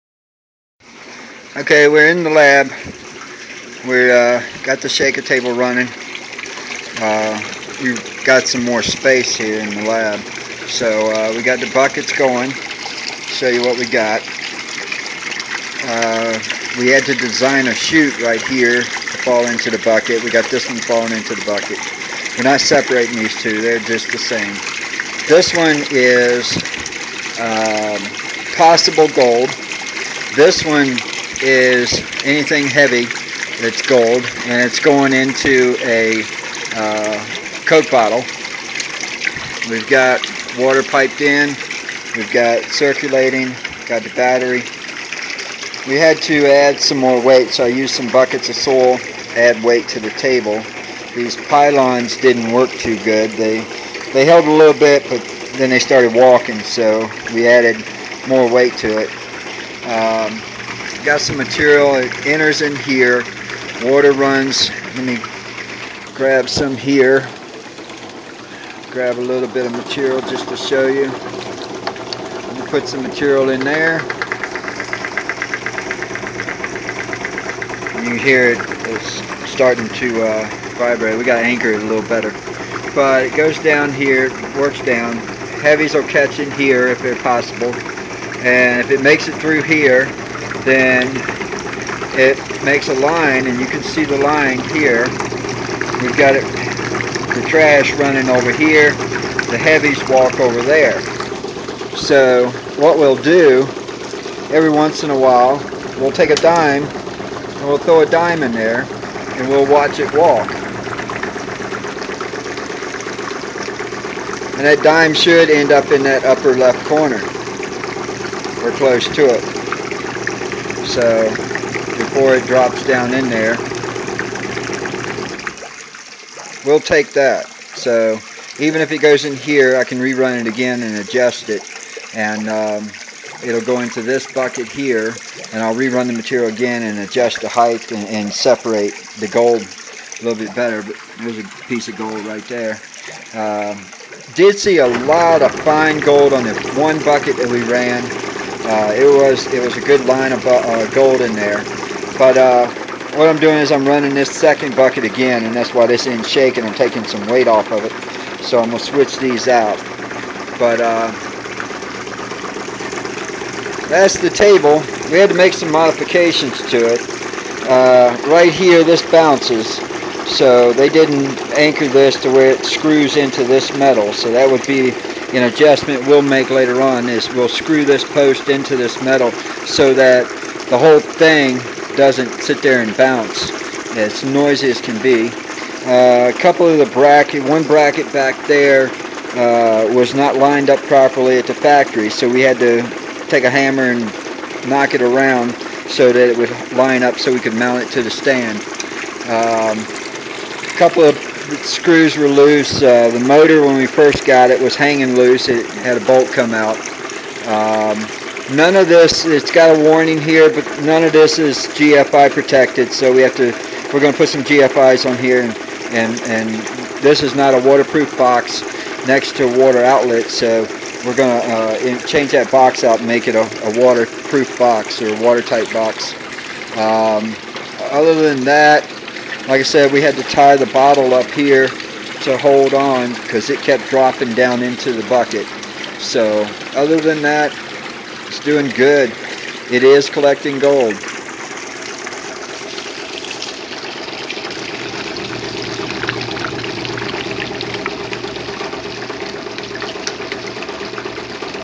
RP-4 Gold Shaker Table - Mountain Mining Corporation
Just set up the RP-4 shaker table. Learning how to use it. It is recovering very fine gold along with other metals. Not what we expected, but it seems to be working.